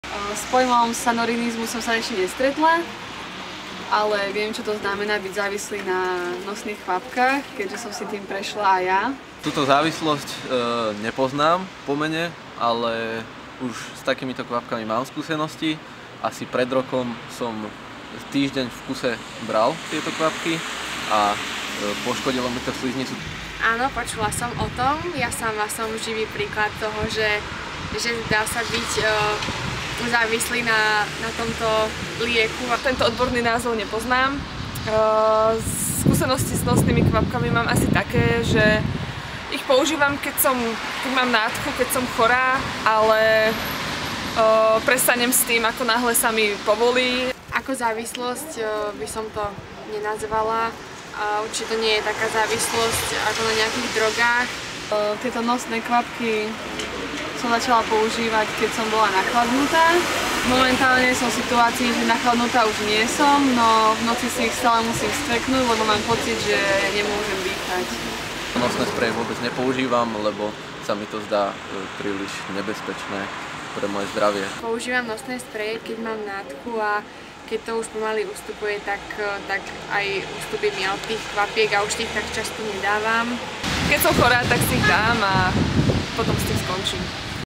Sanorinizmus - čo vie verejnosť o závislosti na nosných sprejoch?
Anketa o sanorinizme pre môj článok, uverejnený na portále munimedia.cz. (Portál provádzkovaný Katedrou mediálních studií a žurnalistiky Fakulty sociálních studií Masarykovy univerzity v Brne)